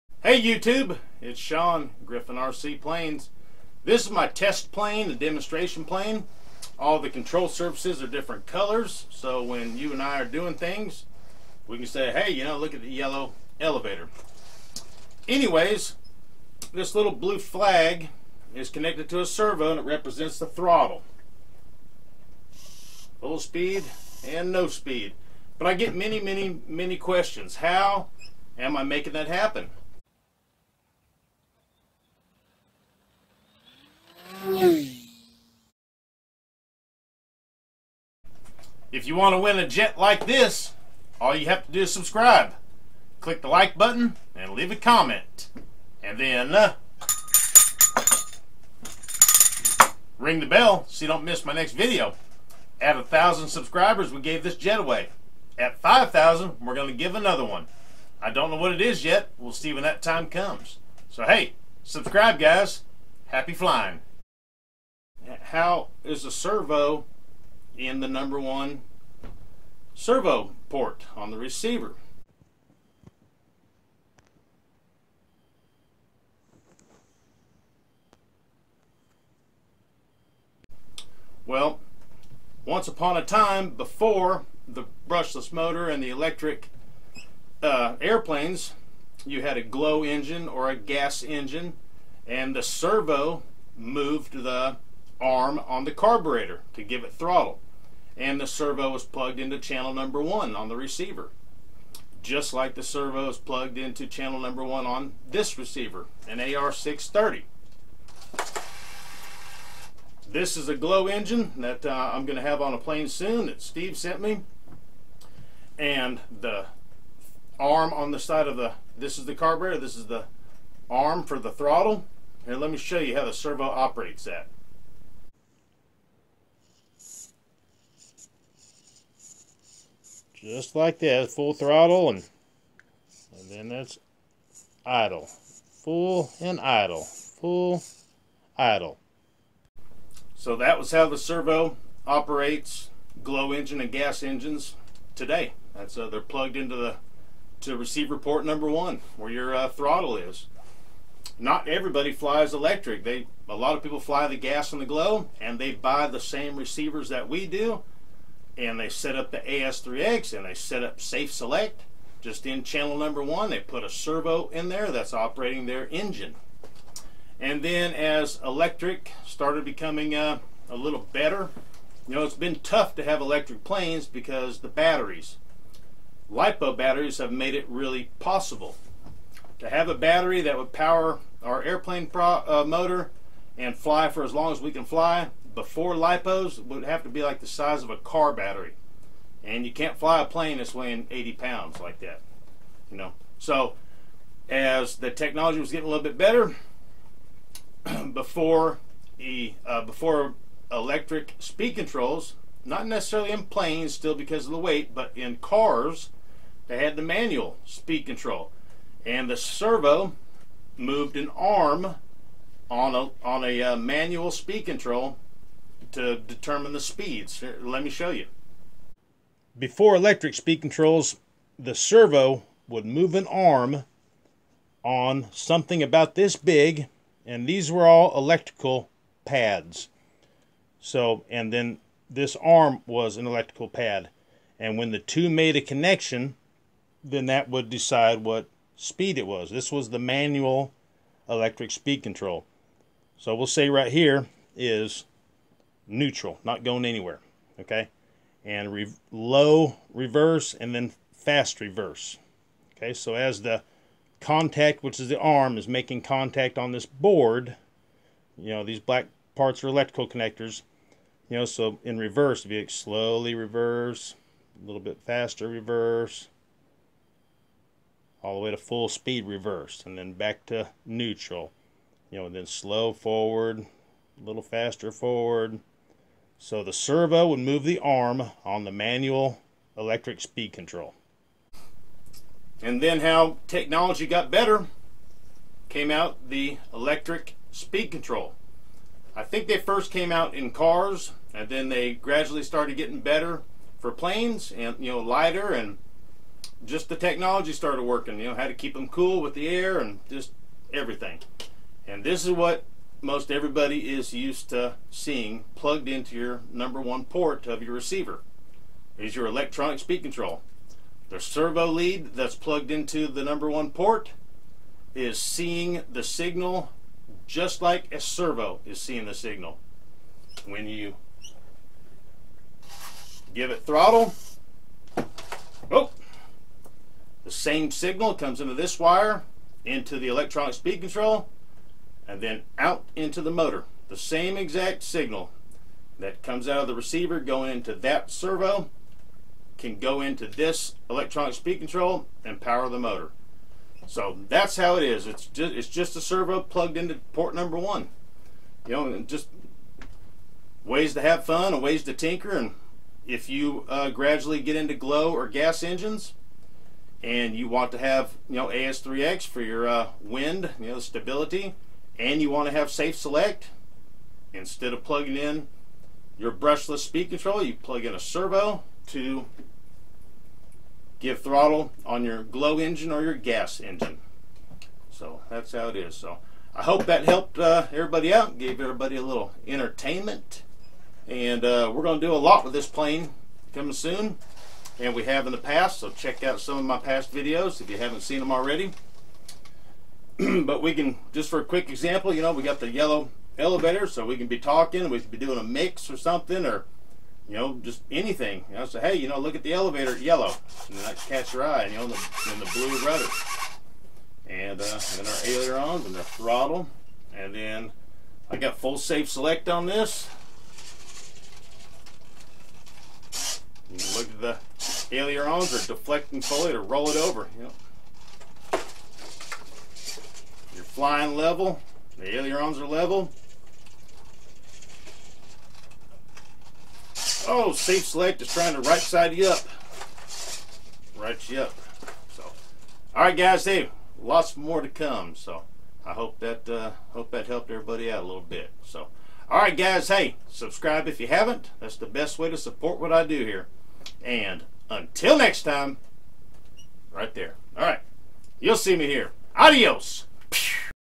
A SERVO FOR THROTTLE INSTEAD OF A ELECTRONIC SPEED CONTROL IN YOUR RC AIRPLANE SPEKTRUM RECEIVER ??
THIS IS HOW AND WHY I USE A SERVO INSTEAD OF A ESC IN MY SPEKTRUM RECEIVER WITH AS3X AND SAFE SELECT AR630 AR631 AR636 AR637TA AR637T AND ALL OF THEM